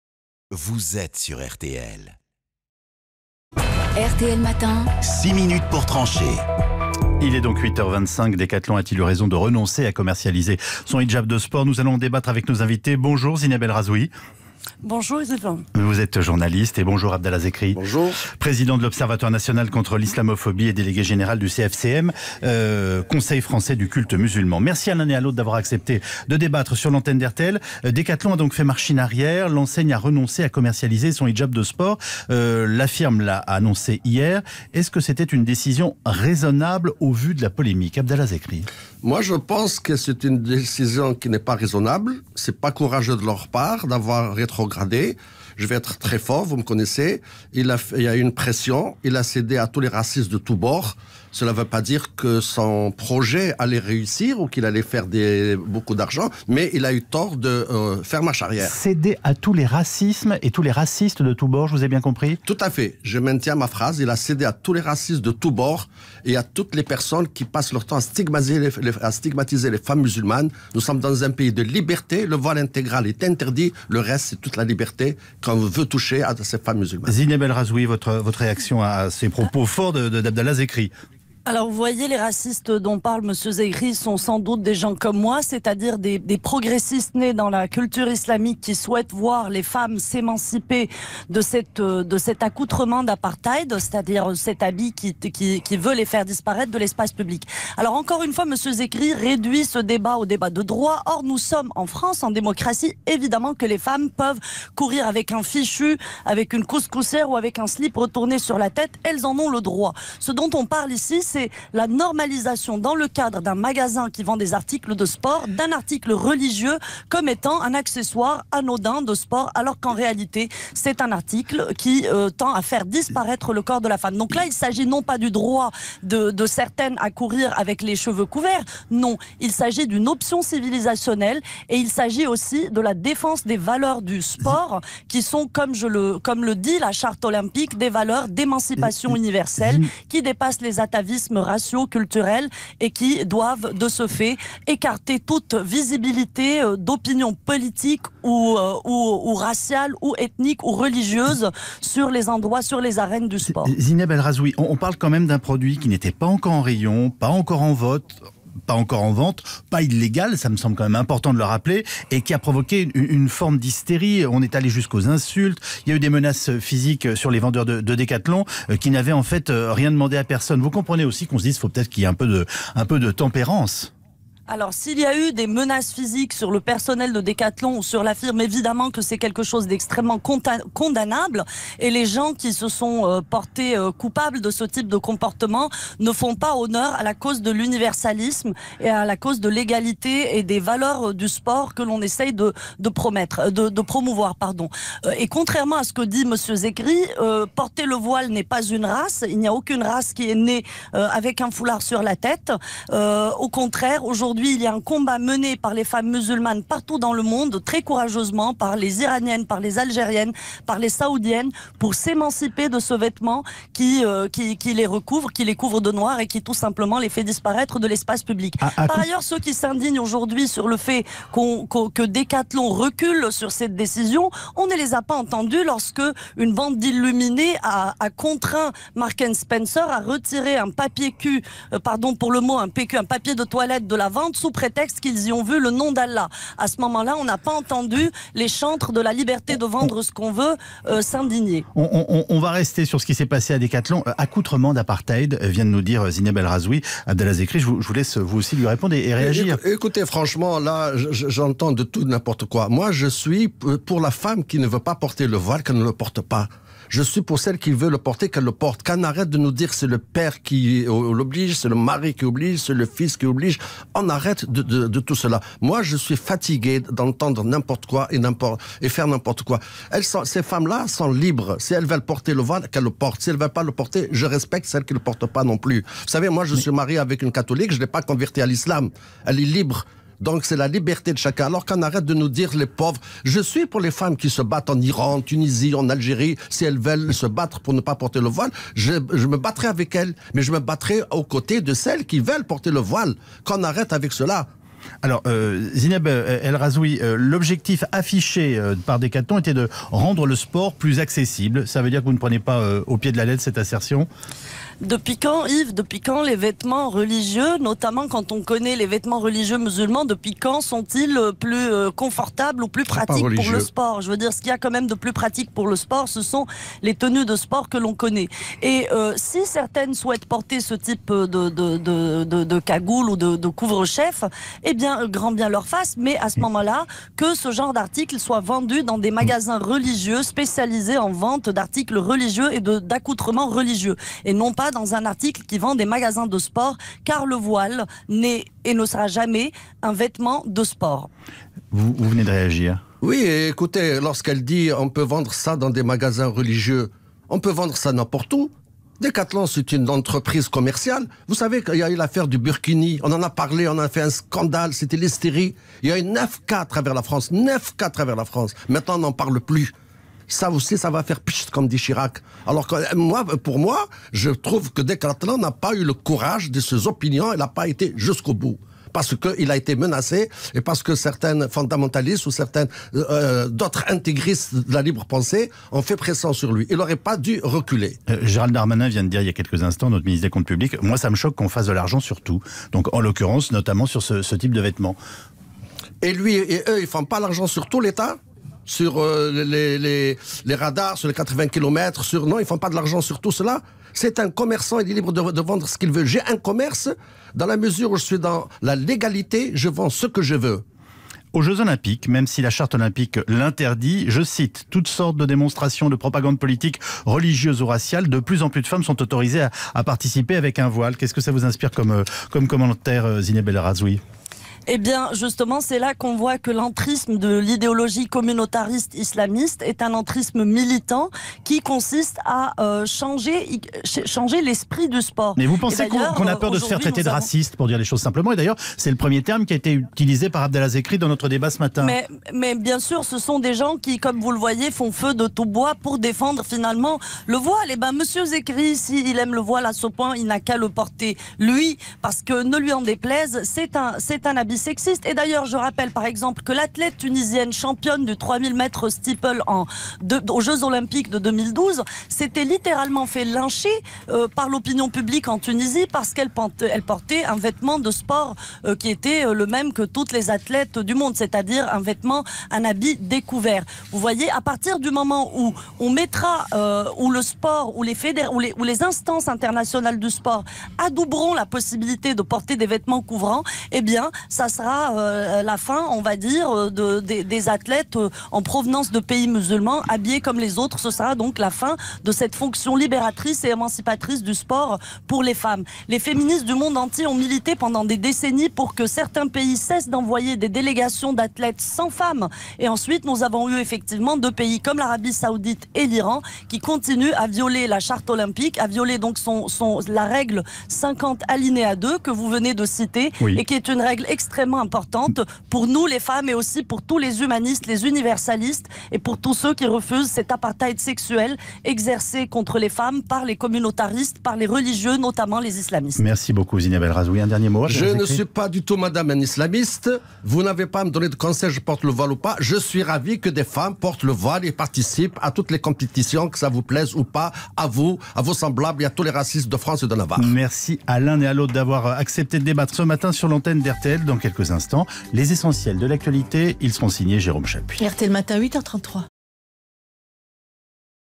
"Hijab de running" : Decathlon "a cédé à tous les racistes de tous bords", dit Abdallah Zekri
INVITÉS RTL - Decathlon a fait machine arrière. L'enseigne a renoncé à commercialiser son hijab de sport. A-t-elle eu raison ? Le président de l'observatoire national contre l'islamophobie Abdallah Zekri, et la journaliste Zineb El Rhazoui donnent leur point de vue.